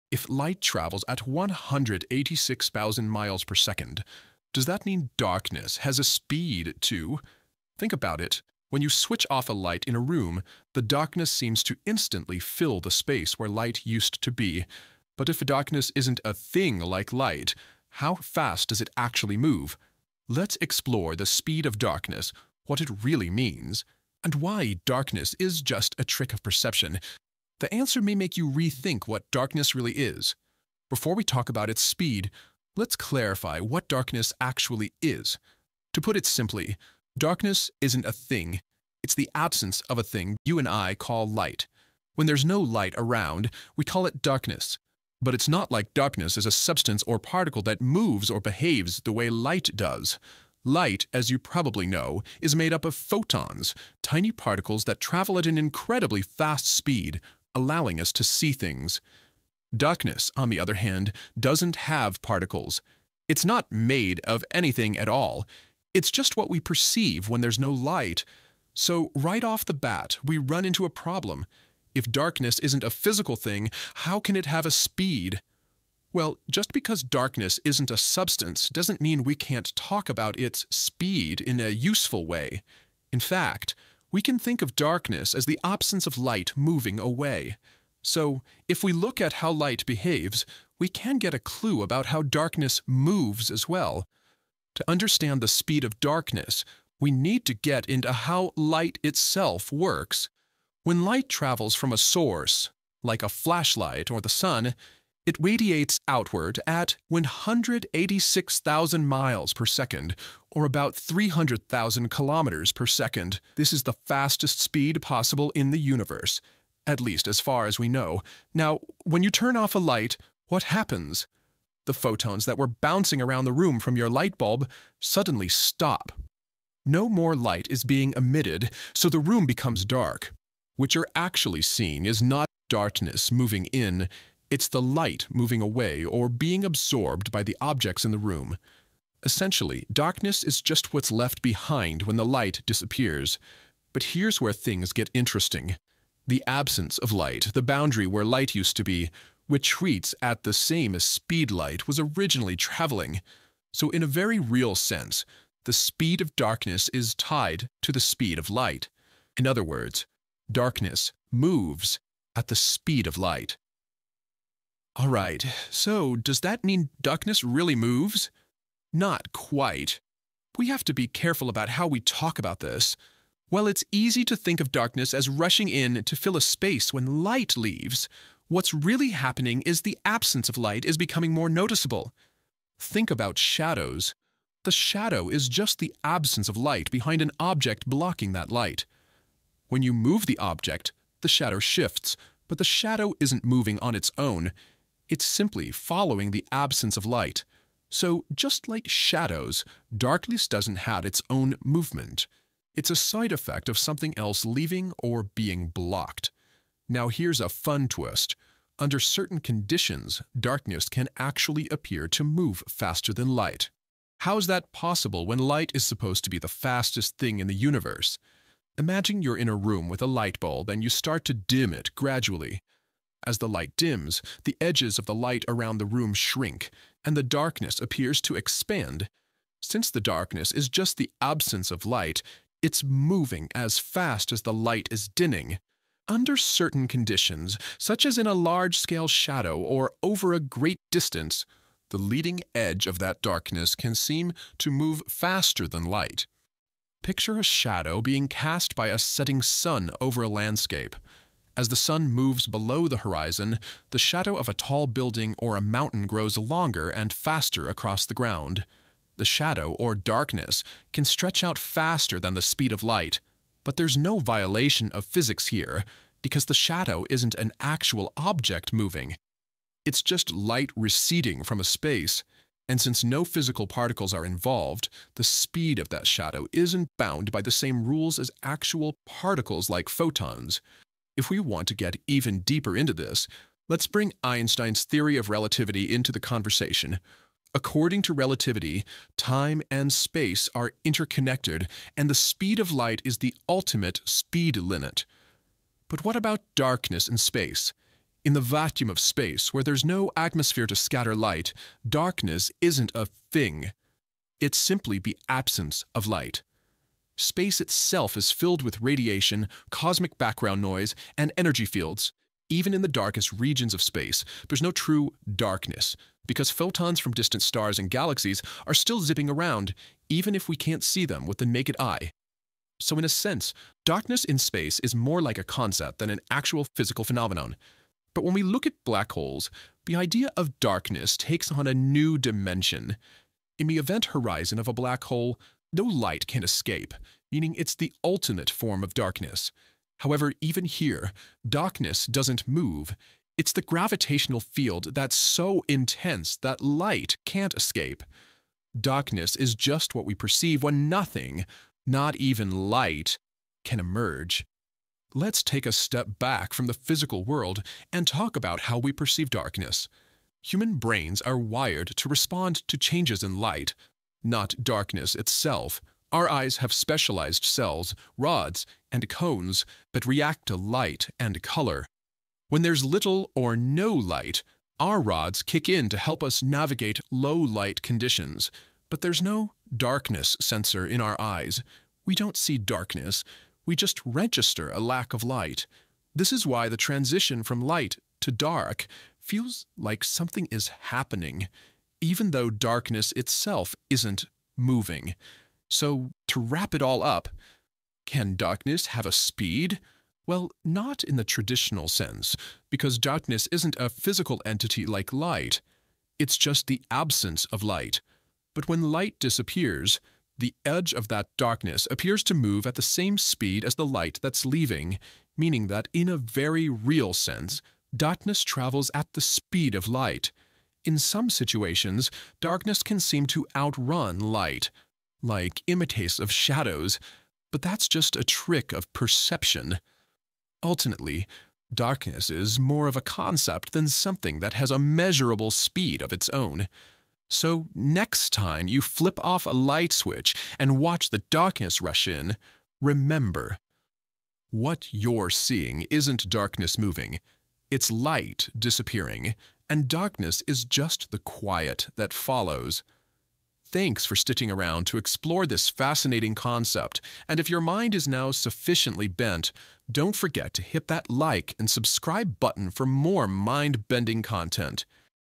The Speed Of Darkness!
"What is the speed of darkness? It's a question that flips our understanding of physics and perception. In this thought-provoking video, we delve into the paradox of darkness and its relationship with light."

Join us as we explore:

The fundamental nature of light and darkness.
How darkness 'moves' in relation to the speed of light.
The phenomenon of shadows moving faster than light without violating the laws of physics.
Philosophical perspectives on darkness as more than just the absence of light.
Practical implications in science and thought experiments.
"Whether you're a science enthusiast, a philosopher at heart, or simply curious about the world around you, this video sheds light on one of the most intriguing questions about our universe."